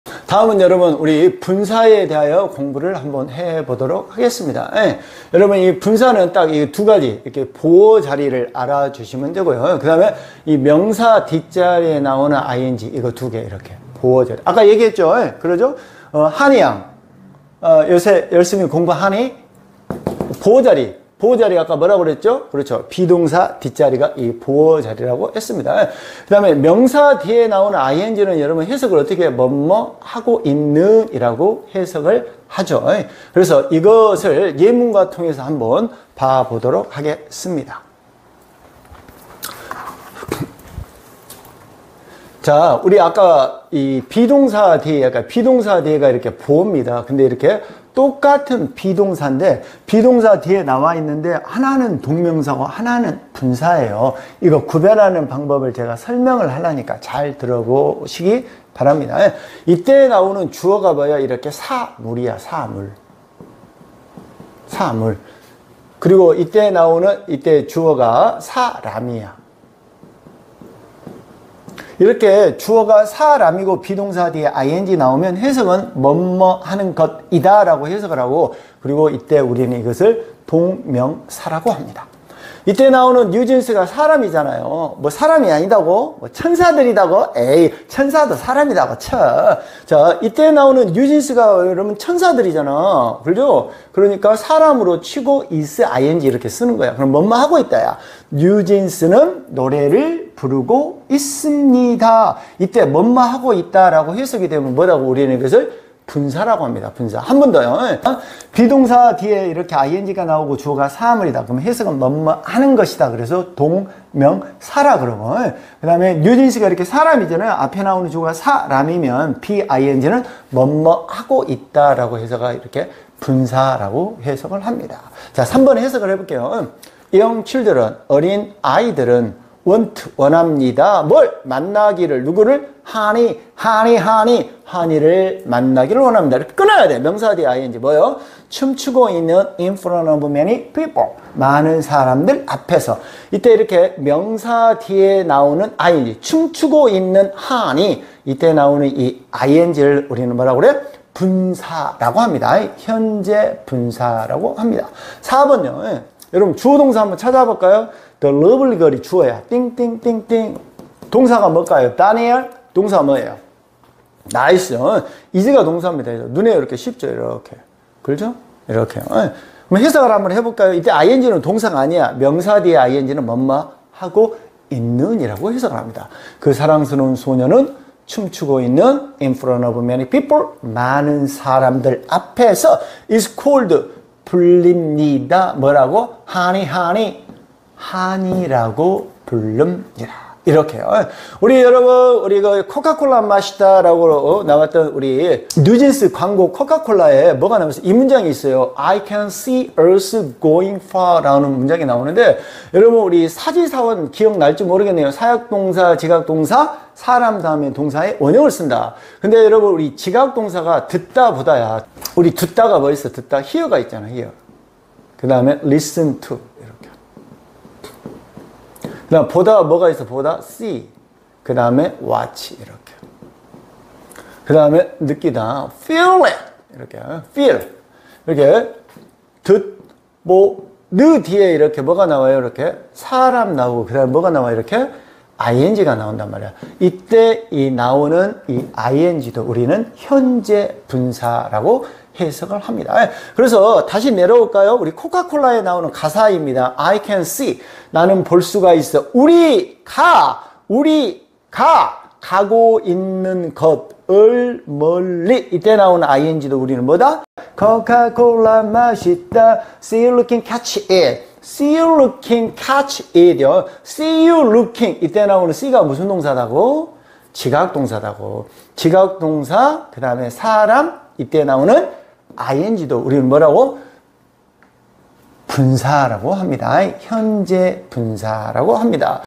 뉴진스와 배워보는 동명사, 분사, 분사구문 구별방법 - 2 분사
동명사에 이어서 분사를 설명합니다.